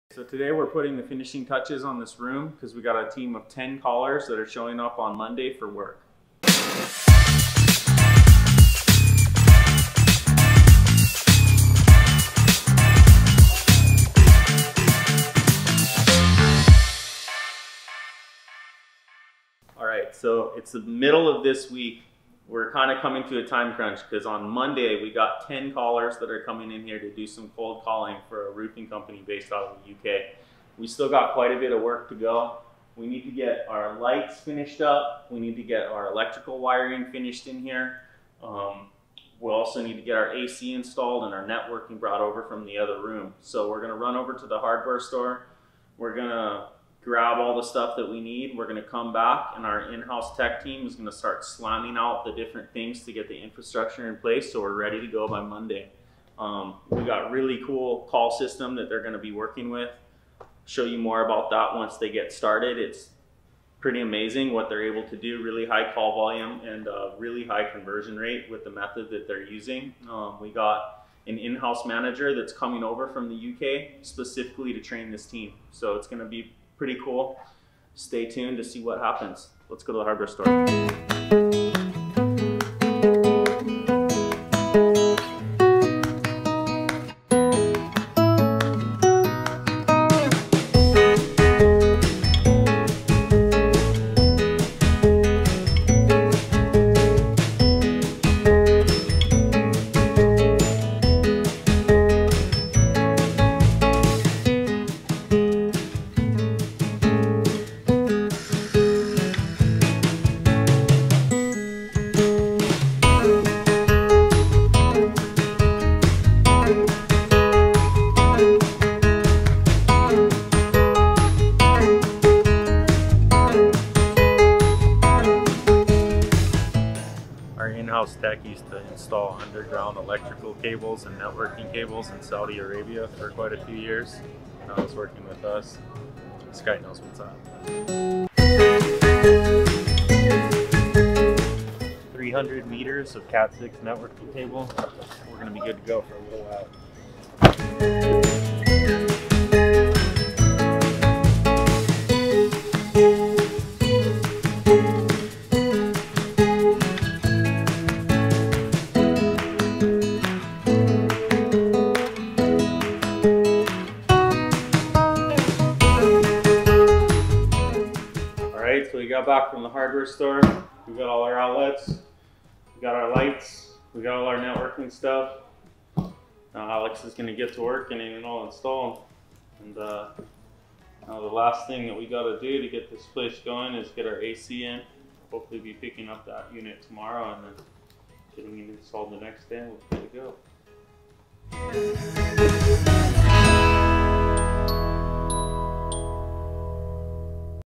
How to wire an Office - Trouble Free VLOG
Want to know how to wire an office? Alright so it's the middle of this week. We're kind of coming to a time crunch because on Monday we got 10 callers that are coming in here to do some cold calling for a roofing company based out of the U.K. We still got quite a bit of work to go. We need to get our lights finished up. We need to get our electrical wiring finished in here. We also need to get our AC installed and our networking brought over from the other room. So we're going to run over to the hardware store. We're going to grab all the stuff that we need. We're going to come back and our in-house tech team is going to start slamming out the different things to get the infrastructure in place so we're ready to go by Monday. We've got a really cool call system that they're gonna be working with. Show you more about that once they get started. It's pretty amazing what they're able to do really high call volume and really high conversion rate with the method that they're using. We got an in-house manager that's coming over from the UK specifically to train this team. So it's going to be pretty cool. Let's go to a hardware store.

All right so we got back from the hardware store we get all our outlets. We've got our lights. We've got all our networking stuff. Now Alex is going to get to work and get them all installed. And the last thing that we've got to do to get this place going is get our AC in hopefully be picking up that unit tomorrow and getting installed the next day.